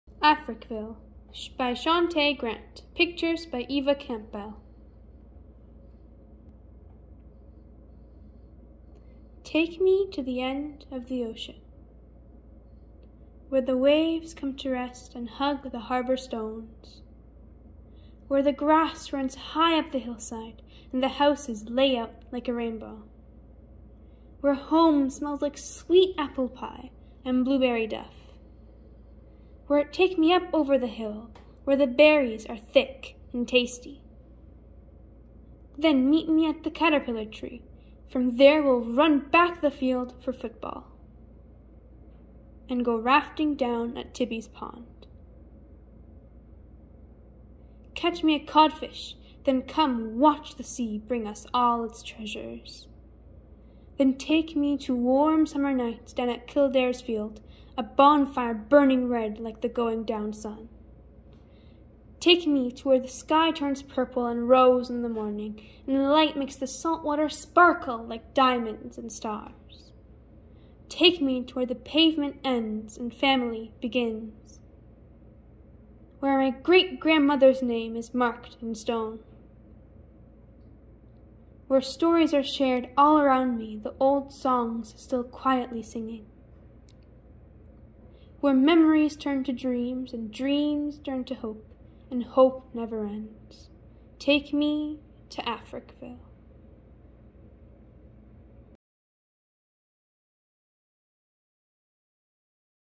Africville by Shauntay Grant *READ ALOUD*
A beautiful story about Africville, NS.